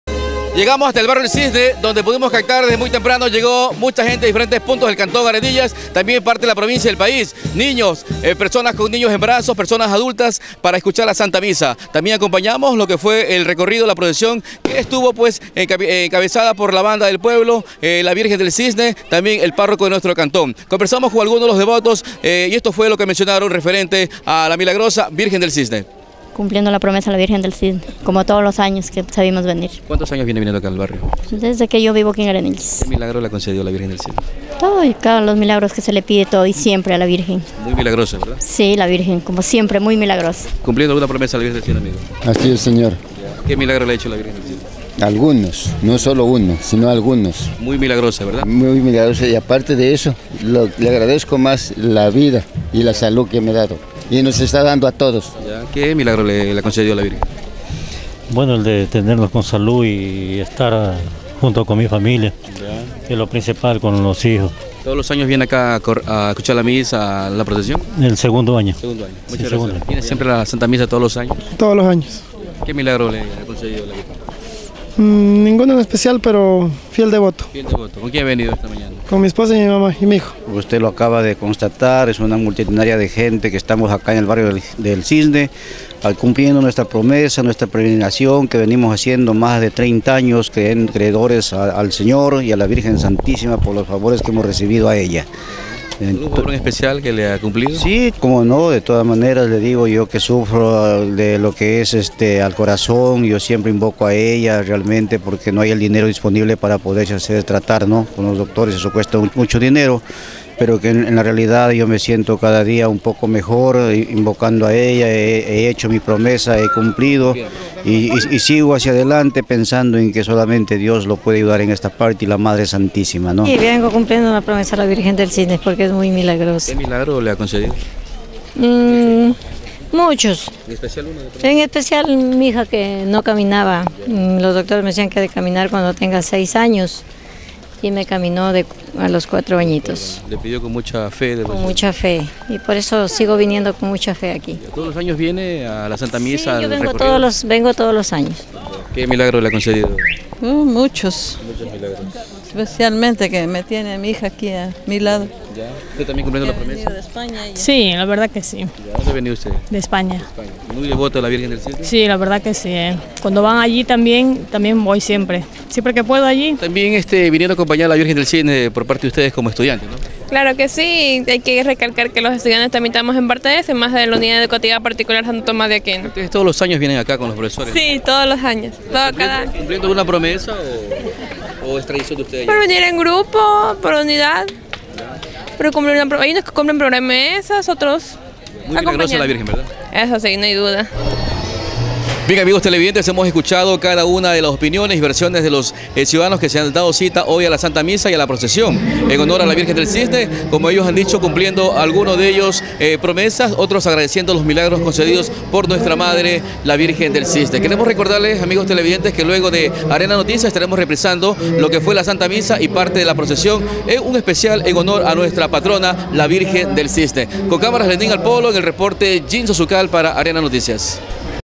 Barrio el Cisne celebro sus fiestas co la Santa Misa y Procesion
Cientos de votos llegaron hasta el barrio el cisne de nuestro cantón para participar de la santa misa y procesión.